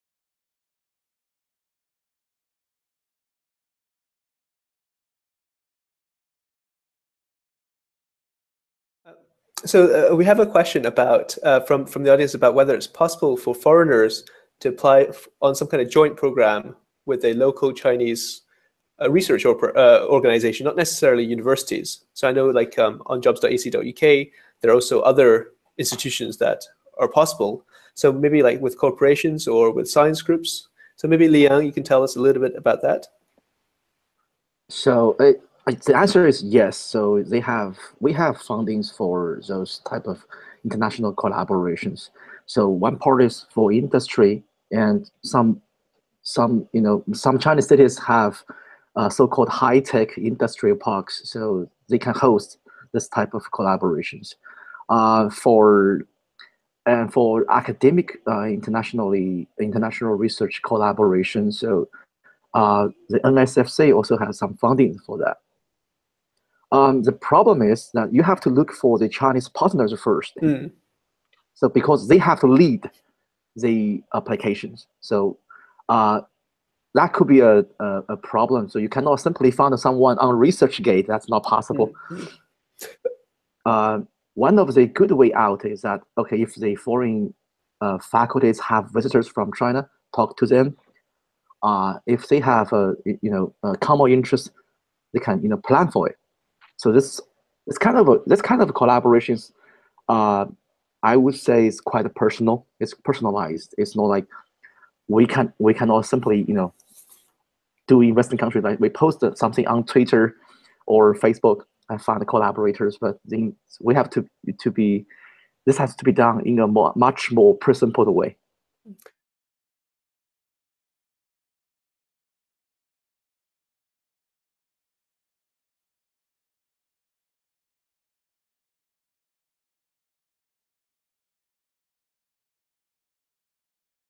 Can I apply to a joint programme with corporations/science groups?
Chinese academics contemplating returning to academia in China are often unsure if it is the right move for them. To find out more about this, jobs.ac.uk recently conducted a focus group with 25 Chinese academics who between them have a wealth of experience, and who are currently working in the UK, to discuss perceptions of returning to pursue their academic career in China. As the result of issues discussed during this focus group, we hosted a 60-minute live video event via a Hangout on Air called ’Working in China’ during the week commencing Oct 23rd, 2017 to address these issues in a wider context.

By watching this jobs.ac.uk live Q&A you will learn about:

- Available funding and how to apply
- Career options available in China
- Working conditions in China
- Research publication expectations
- Options to lead your own research in China

We have picked a panel of experienced, passionate and friendly experts all keen to help answer your questions.

Shengqiang Chen

Shenqiang Chen is currently an academic visitor at the School of Law, University of Sheffield and got his PhD degree in 2011 at the Law school of Wuhan University in China. He then went on to Henan University, China and is now an Associate Professor on Public Law. Shengqiang's research interests are constitutional law, administrative law and legal history.

Huaiyi Huang

Huaiyi Huang received his bachelor (2011) and PhD degree (2016) under the guidance of Prof. Liangnian Ji and Prof. Hui CHao at Sun Yat-Sen University (China). During 2015–2016, he conducted a one-year exchange PhD project on photodynamic anticancer therapy under the guidance of Prof. Gilles Gasser at University of Zurich (Switzerland). During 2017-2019, he works as a Newton research fellow supported by Royal Society in Prof. Peter Sadler’s group in University of Warwick. His research focuses on the chemistry of metals in medicine (bioinorganic chemistry, chemical biology and medicine). In particular, on the design and mechanism of action of therapeutic metallic complexes, including metallic photoredox catalysts, organometallic anticancer complexes, photoactivated metal complexes, metalloantibiotics.

Le Yang

Le Yang received his B.S. and M.S. degrees from the University of Electronic Science and Technology of China (UESTC) in 2000 and 2003, respectively, both in Electrical Engineering. Since August 2004, he has been a research assistant with the University of Victoria, Victoria, BC Canada, working on diversity techniques for advanced wireless communications. In 2006, he was a research assistant with the McMaster University, Hamilton, ON, Canada, focusing on computational modeling of neural substrates for tinnitus. Since January 2007, he has been a PhD candidate at the University of Missouri, Columbia, MO USA, working on passive localization techniques. He received his PhD degree in Electrical and Computer Engineering in December 2010 from the University of Missouri. Since June 2011, he has been an associate professor at the Jiangnan University, Wuxi, China. He has published extensively in the areas of statistical signal processing, smart transportation, neural networks and machine learning. In 2017, he received the Tammy Blair Best Student Paper award from the International Conference on Information Fusion (FUSION) during his academic visit at the University of Sheffield, Sheffield, UK.